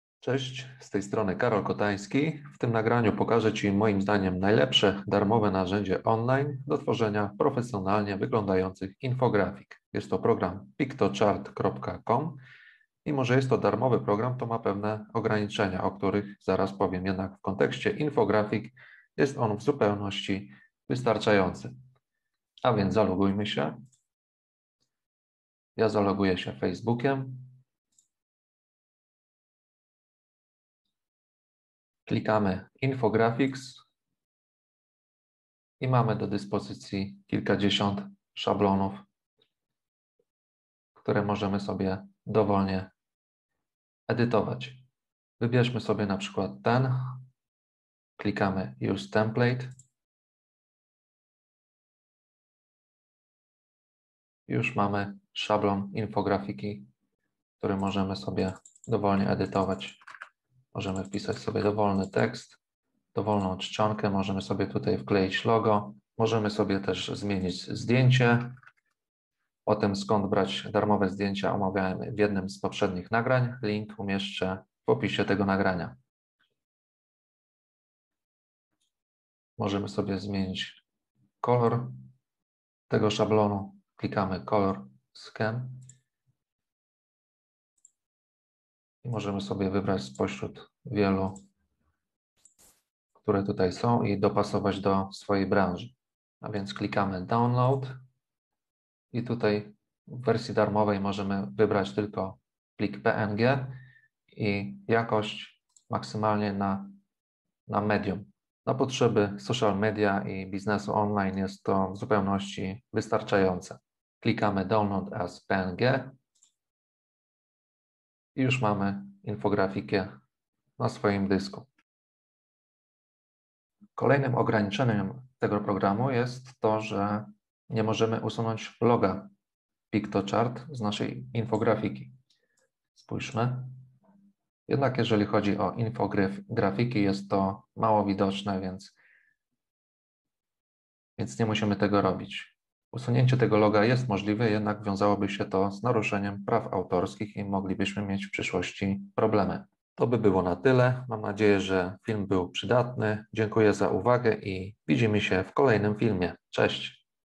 Jak robić infografiki. Szybko i łatwo
W nagraniu pokazuję najlepsze, darmowe narzędzie do tworzenia infografik.